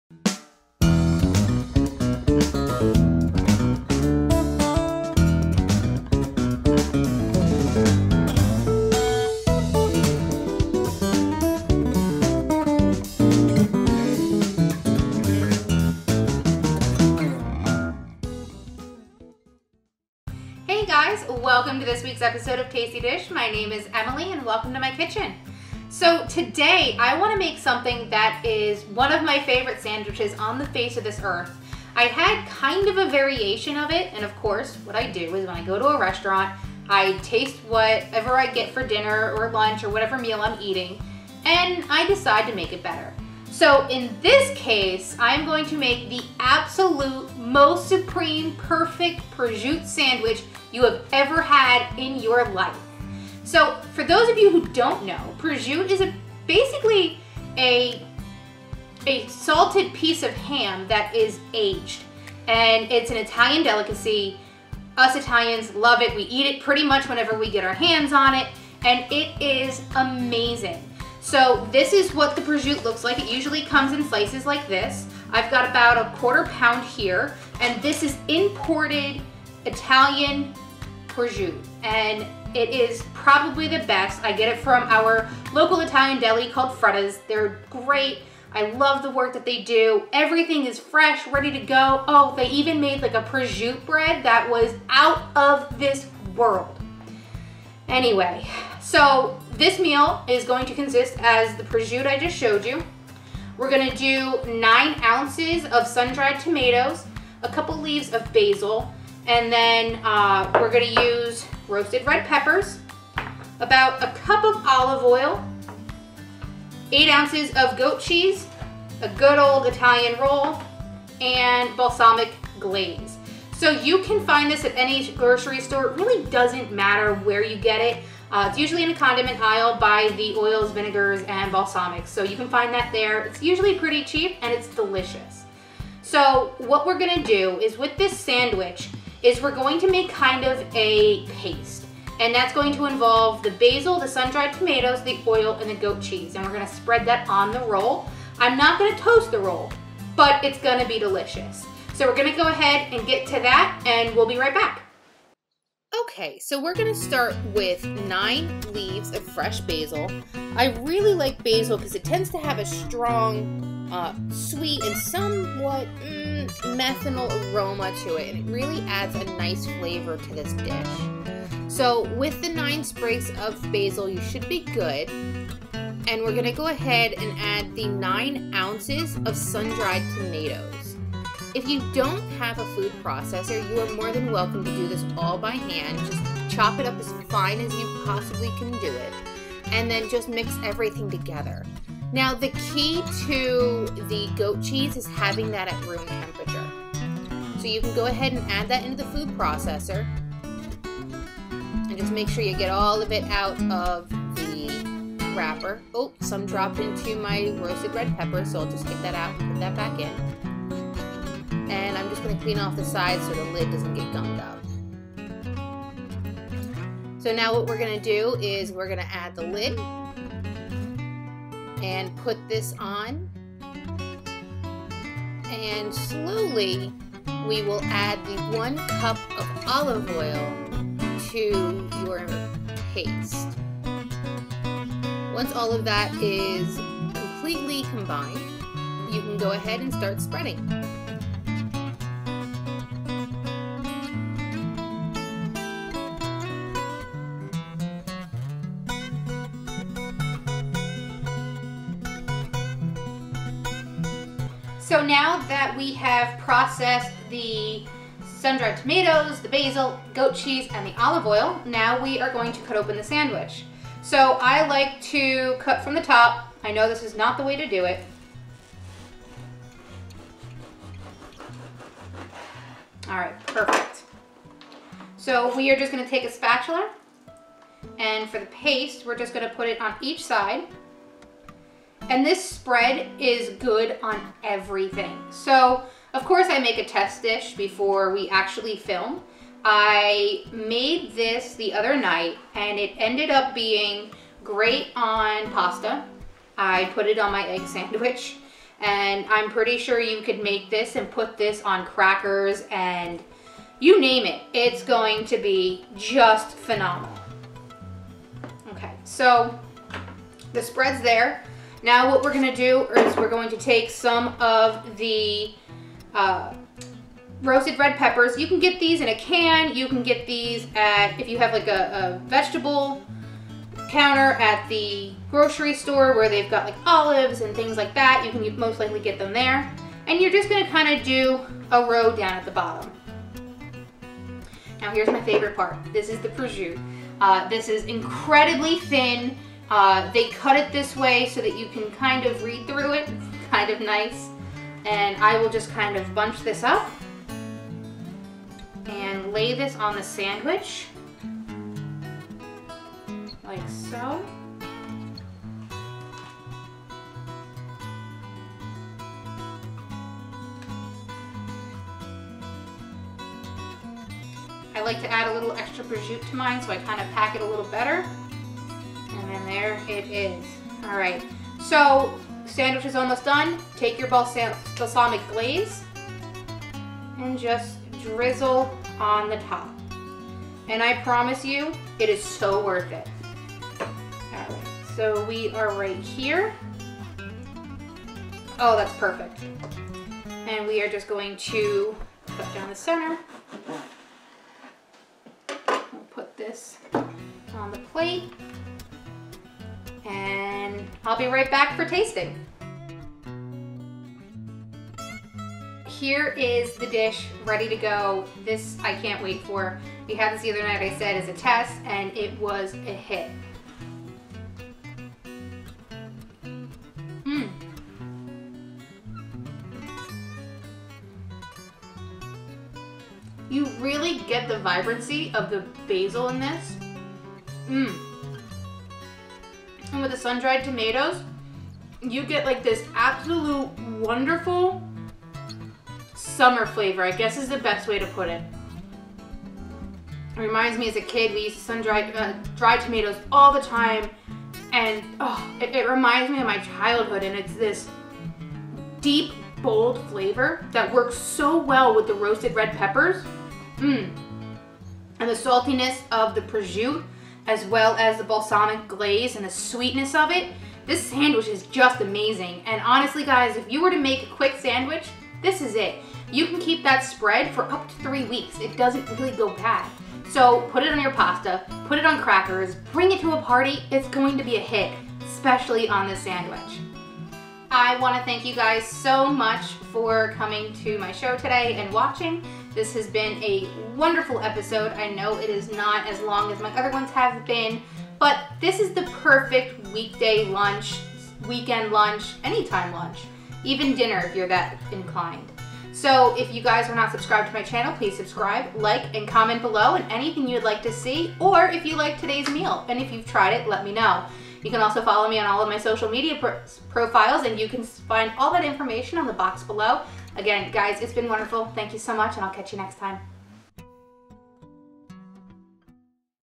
Tasty Dish ep, 4 Supreme Proscuitto Sandwich
This Sandwich is So easy to make, it will be a goto for your hot summer days!

Ingredients:
9 oz Sundried tomatoes (not in olive oil)
9-10 Leaves of Fresh Basil
8 oz Room Temperature Goat Cheese
1 cup of olive oil
1/8 lbs of prosciutto ( just makes one sandwich, you might need to double the recipe for more sandwiches.)
16 oz jar of roasted red peppers
Any kind of Bread you prefer, I went with an Italian Hoagie Roll.

there will be leftover spread, and you can pretty much put it on anything you wish!

In a food processor, add the 9-10 leave of fresh basil, 8 oz of sundried tomatoes, the room temperature goat cheese and start the food processor, while it's blending add the 1 cup of olive oil to the spread and allow it to get fully incorporated.

Once done, its time to assemble the sandwich.

Smear the spread on both sides of the bread, add the roasted red peppers to either side of the sandwich. Add the prosciutto as you wish, use the balsamic glaze and drizzle it on top of the prosciutto. Put the sandwich together and enjoy!

Snapchat (for all other fun including food, friends, animals and my life): Crosser41413